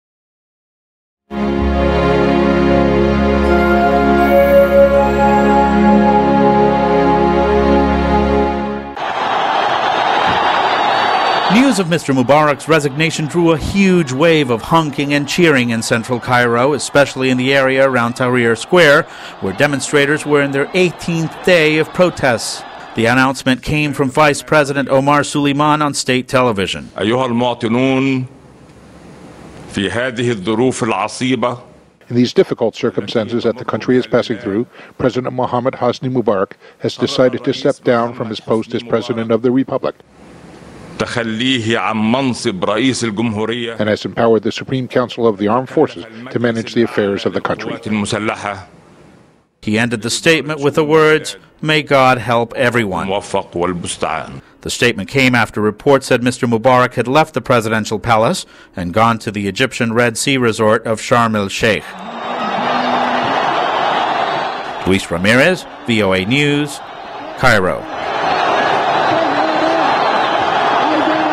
Egyptian President Hosni Mubarak Resigns
After 18 days of protest, Egyptian President Hosni Mubarak has stepped down after ruling the country for nearly three decades.  VOA's Luis Ramirez reports from Cairo.